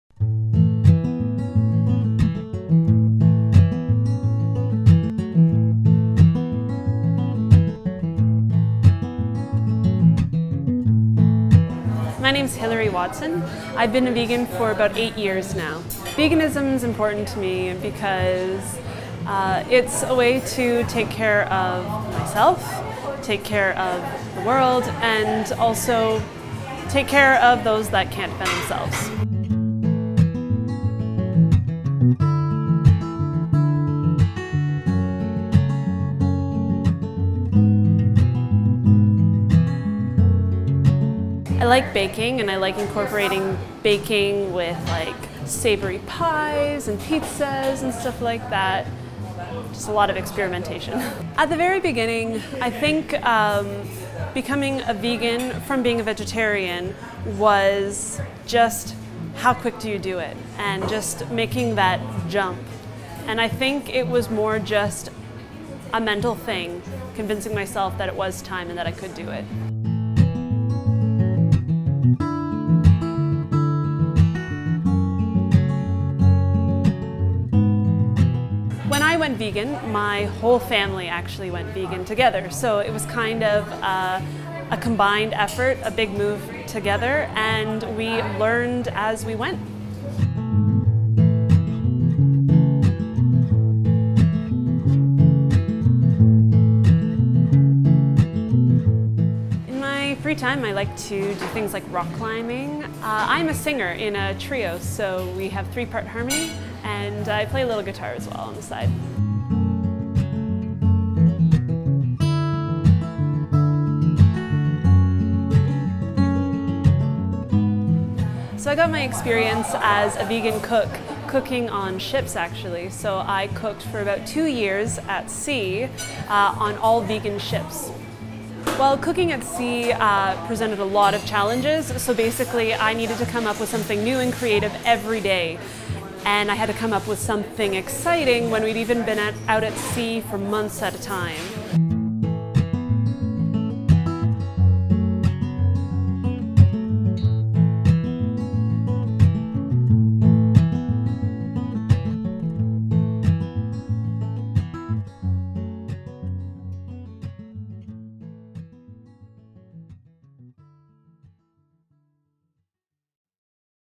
Changing Dietary Lifestyles - Vegans of PMK
We talked with Plant Matter Kitchen's  Hillary Watson about why veganism is important to her and how it has impacted & influenced her lifestyle.